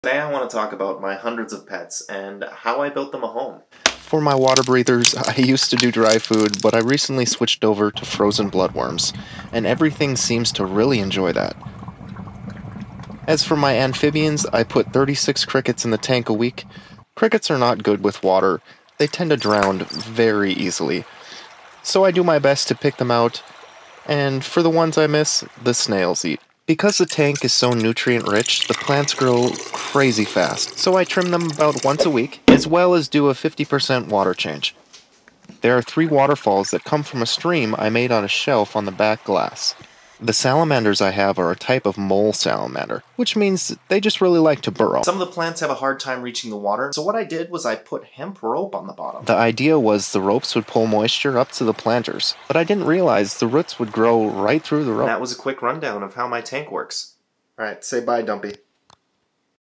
Dumpy the giant tree frog.
This is my care guide for my DIY Paludarium ecosystem. We go through everything between white tree frog care tips to tiger salamander feeding.